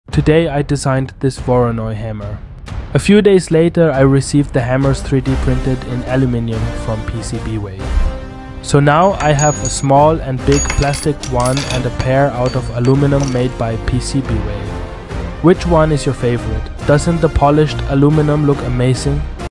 I designed a HAMMER and 3D Printed it in METAL! 🤯
I thought i will try to make something different today... 😂😍

What do you think about this model and what should i make next?

You can find the 3D Printing Files, Models and used Filament (also in my Bio) 👍🏽